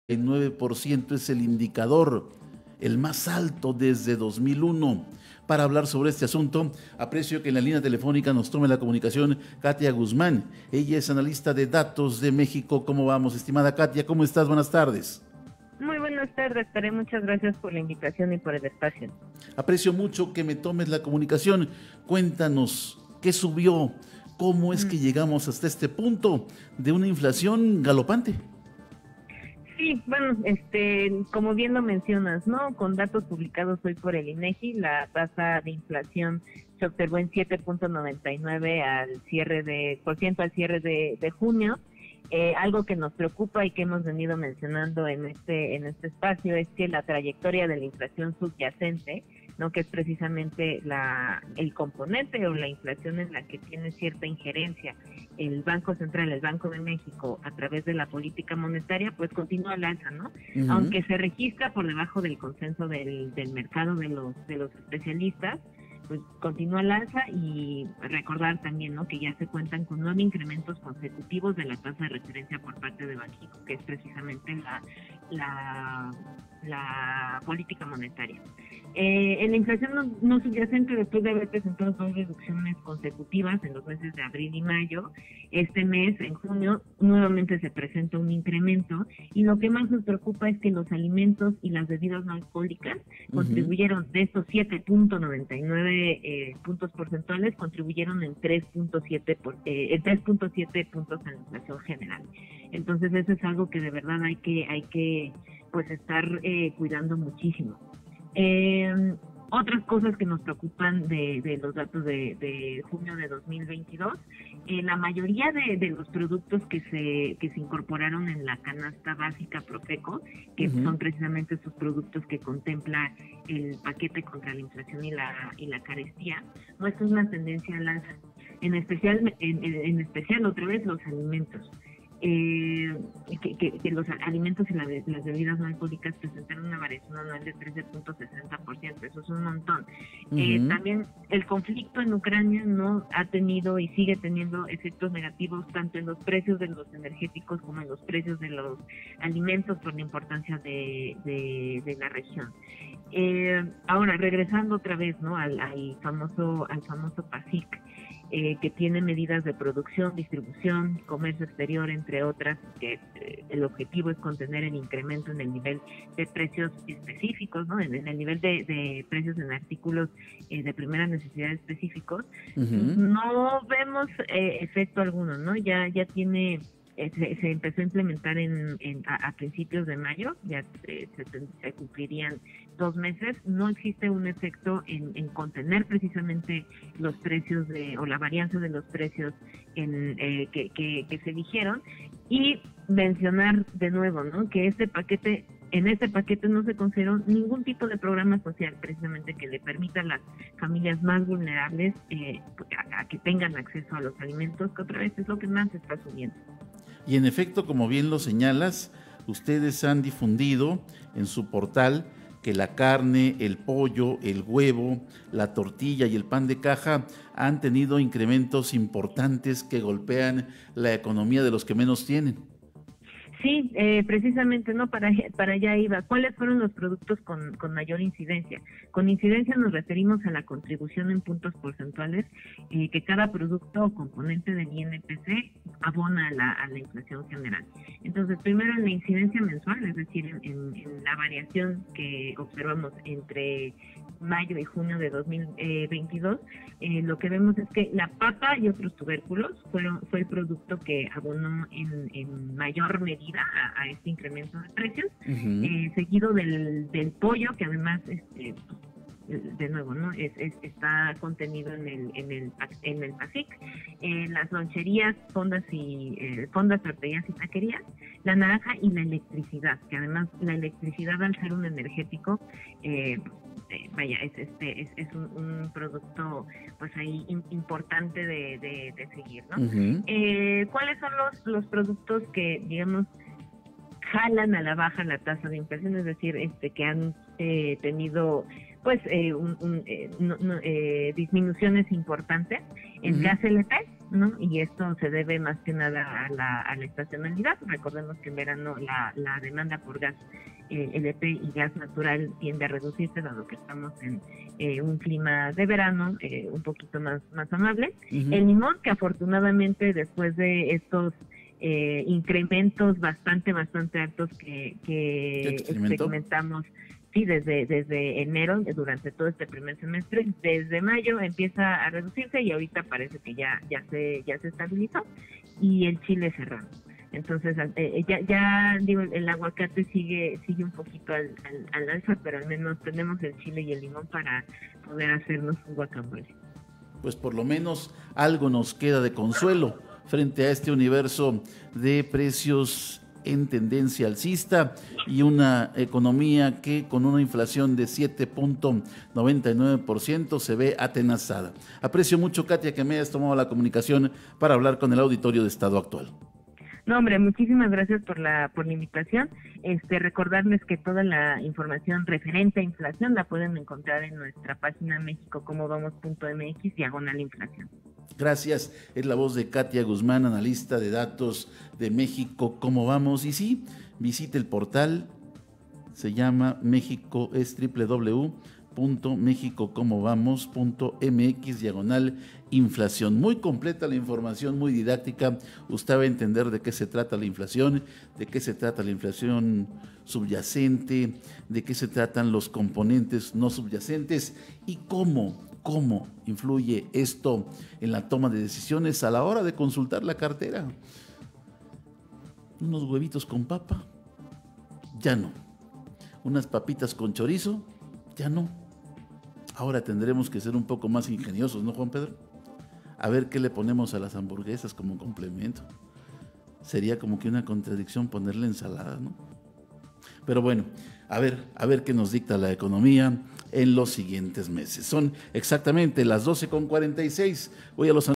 Alimentos básicos tienen una tendencia al alza
En entrevista para Estado Actual, Katia Guzmán, coordinadora de datos en México, ¿cómo vamos?, especificó la situación de la inflación y cómo afecta en la economía de las familias.

Noticias de Oaxaca. Estado Actual. estadoactual.com